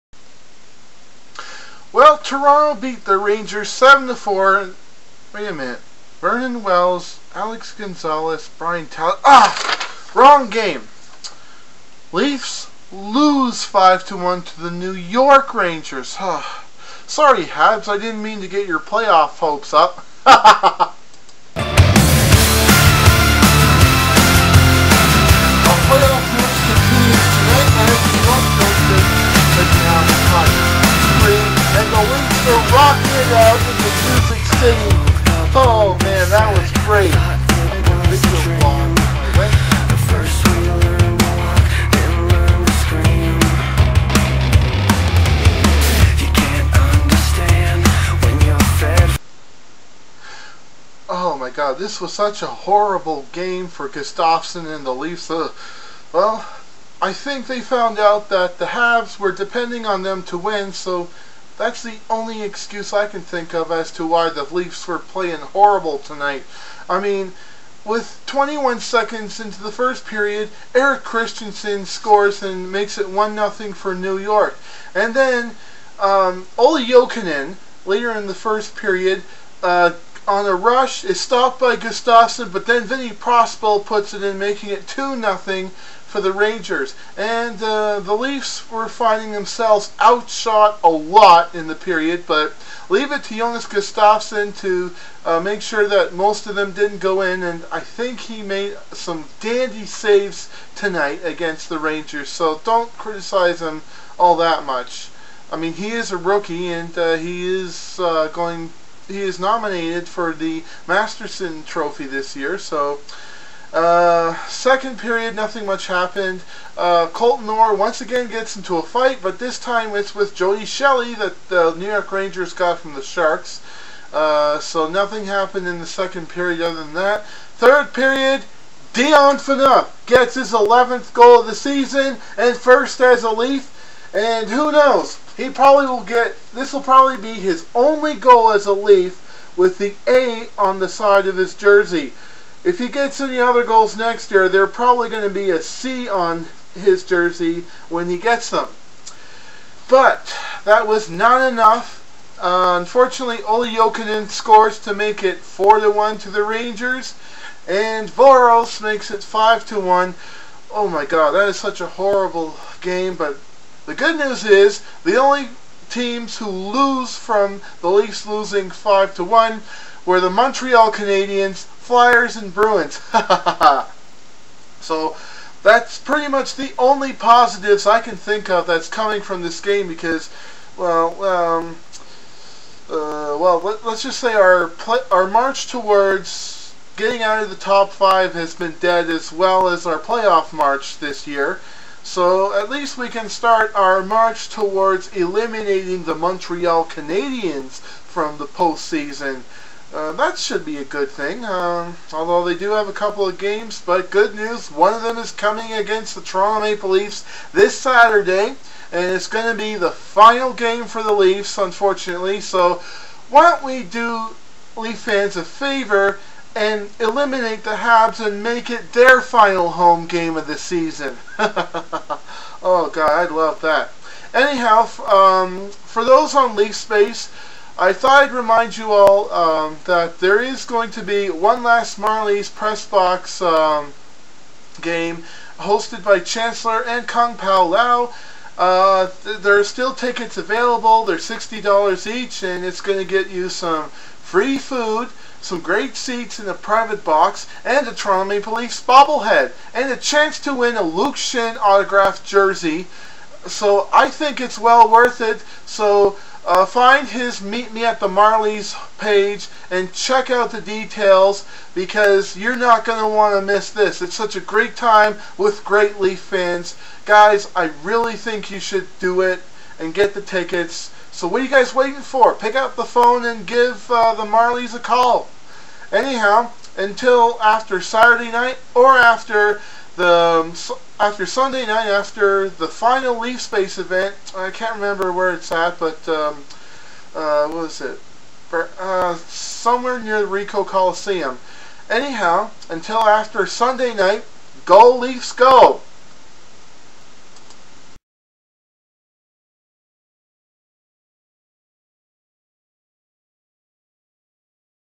LEAFS LOSE 5-1 TO THE RANGERS! .wmv
Don't worry Habs fans. The Habs will still be eliminated this year before the playoffs start :o)